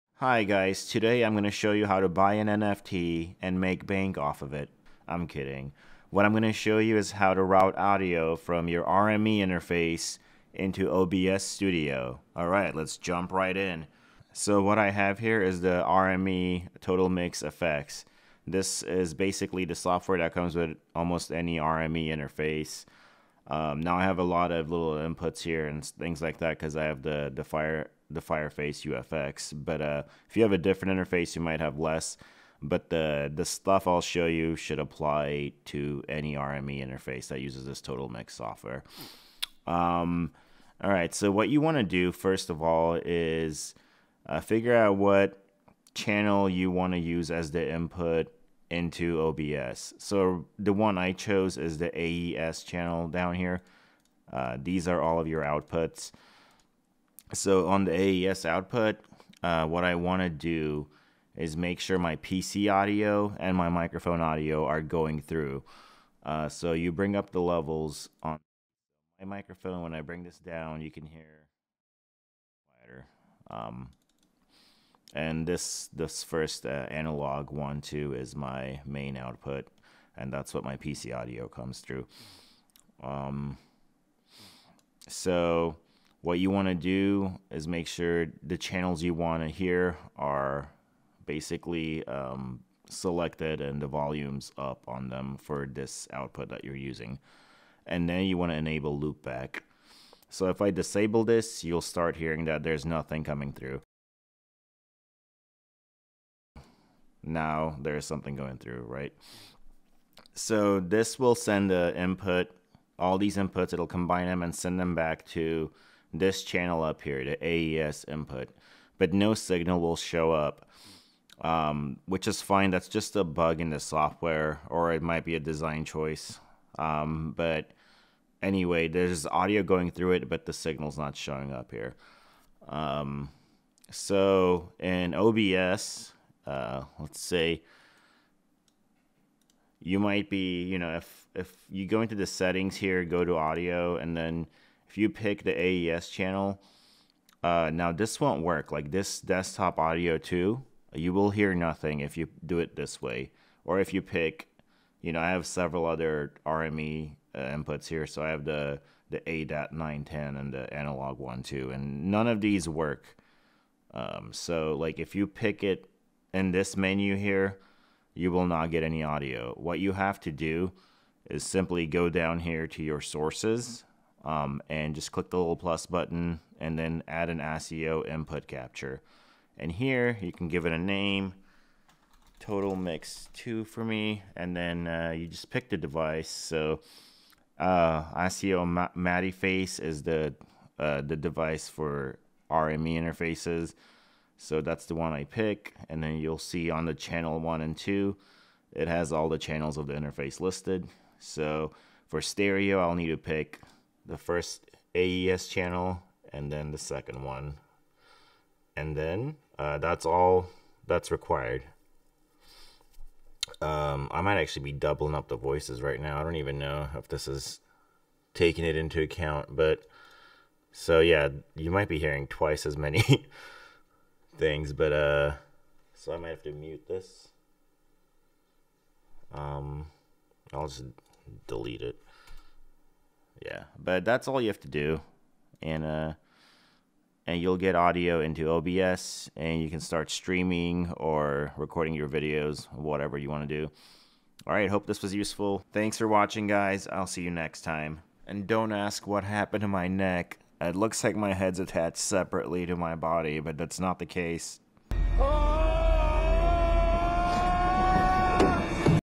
Stream your DAW / PC sound with OBS Studio + RME Interface
I show you how to stream audio from your RME interface to OBS Studio.